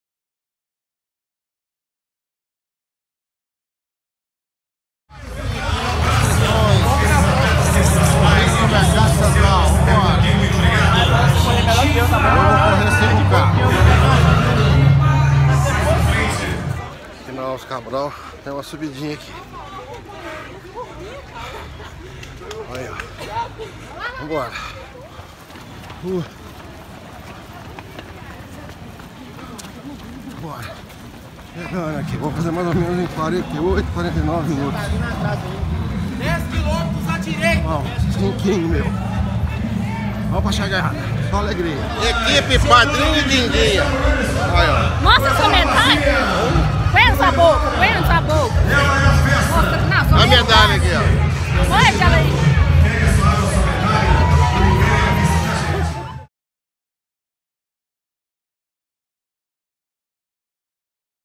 Corrida Mercado Central 2019 - Jun/19 - #9
Corrida Mercado Central - Jun/19.
Mais uma corrida de 5k para minha saúde! Esta é a 12 em 1 ano!!! Top demais!